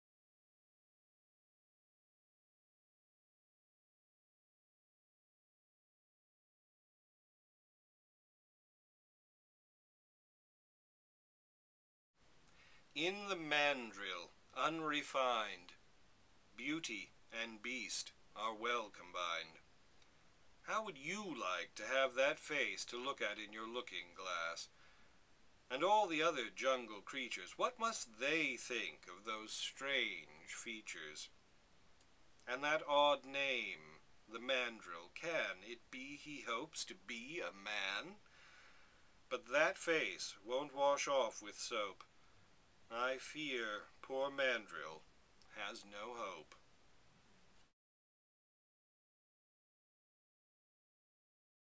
The Mandrill
A poem by Conrad Aiken, read aloud by Brad Craft.